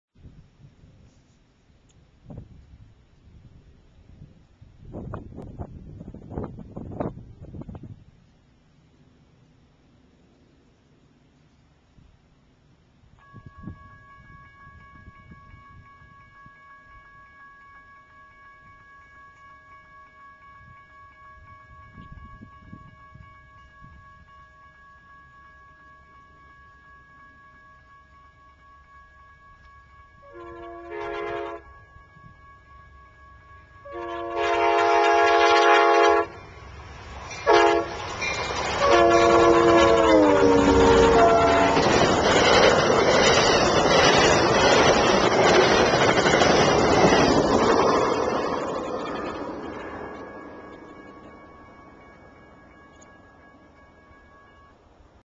Amtrak Southwest Chief Full Speed in Galisteo, NM
Amtrak's westbound Southwest Chief is running on-time today. As it makes its way through Galisteo, NM. This train is going full speed as it heads west towards its next station stop of Albuquerque, NM.  Recorded on December 10, 2011.